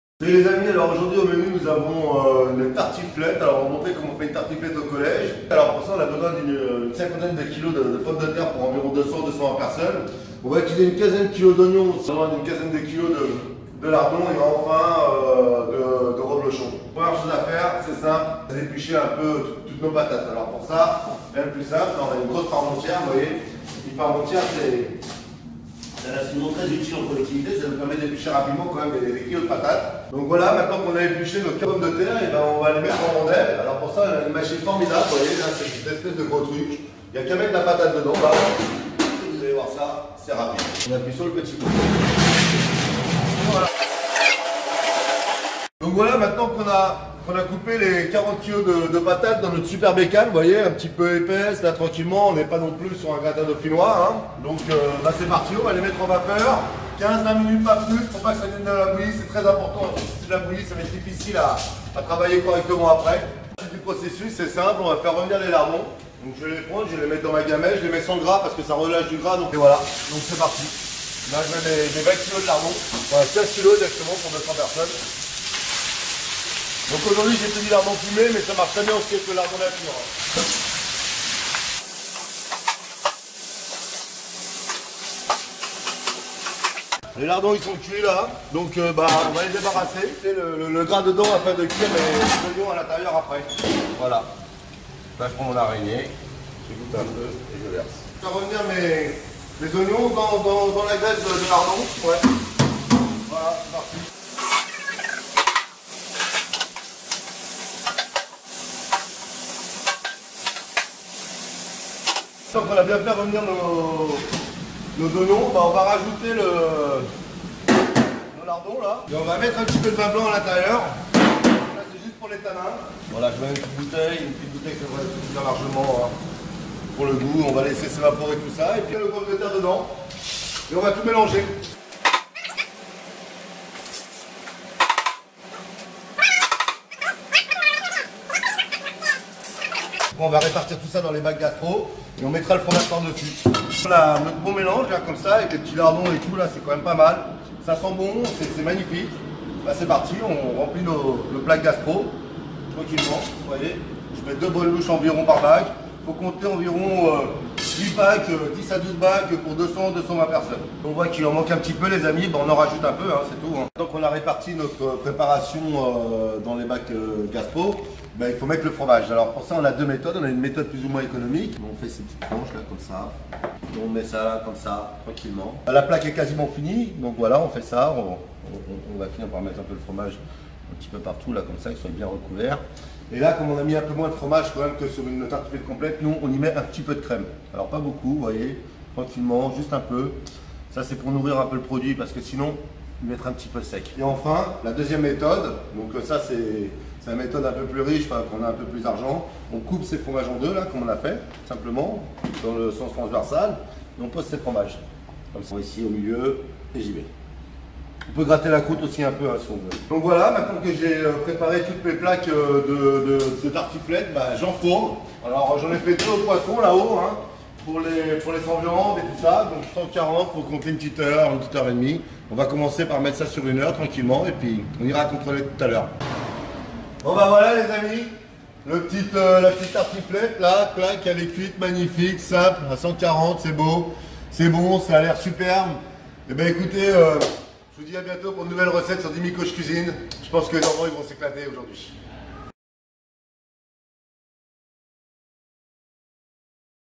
tartiflette pour cuisine collective
pour 220 à 240 personne
50kg de pomme de terre
15 kg de lardons fume ou pas
15 kg d'oignon émincé
25-30 reblochon pour 10 a 12 plaque gastro
facultatif vin blanc et/ou creme liquide

Cuire les pomme de terre en rondelle au four vapeur.

Faire revenir les lardons, débarrasser, puis faire suer les oignons dans la graisse des lardons. Remettre les lardons dans les oignons. Y mettre  un peu de vin blanc facultatif et laisser évaporer. Ajouter les pommes de terre. Mettre le tout dans des bacs gastro gn 1/1. Repartir le reblochon soit en demi, soit en lamelle. Cremer facultatif cuire au four à 140° durant une bonne heure. Je préfère les températures intermédiaires que de a 180°

1kg de pomme de terre
200 gr de lardons préférence fumés
200 gr d'oignon émincés
1 reblochon
Un peu d'huile
Sel poivre

Cuire les pommes de terre avec la peau. Les éplucher et les couper en lamelle d'environ 1/2 Cm
Faire suer les oignons puis les lardons on peut déglacer au vin blanc facultatif

Dans un plat à gratin mettre les pommes de terre en rondelle. Puis les oignons et lardons et enfin recouvrir de pomme de terre. Et la on y met le reblochon soit en rondelle soit en lamelle